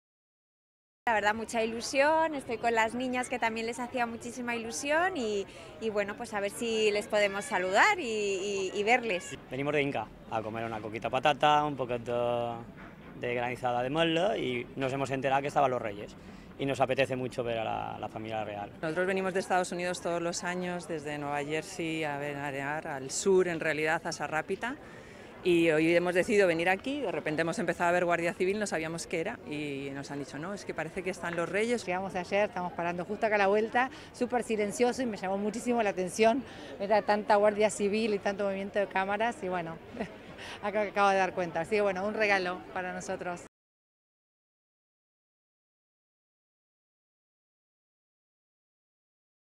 Valldemossins comentant la visita de la família reial
La família reial ha visitat avui horabaixa la Cartoixa de Valldemossa. Els reis Felip i Letícia han estat acompanyats per la princesa d’Astúries i la infanta Sofia en la que és la primera aparició al complet de la família durant les seves vacances d’estiu a Mallorca. Els reis i les seves filles han arribat puntuals en aquesta visita en la qual han pogut visitar aquest històric conjunt monumental del segle XIV.